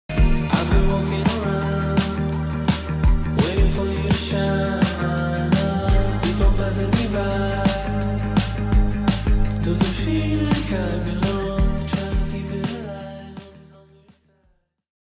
Furla Eyewear SS22
Qualidade, criatividade, jovialidade e contemporaneidade. Esta coleção dedica-se à mulher cosmopolita, urbana e chic. Cosmopolita porque viaja e incorpora um gosto internacional; urbana e chic porque combina roupa e acessórios de forma original, através de combinações arrojadas e de uma extrema atenção ao detalhe.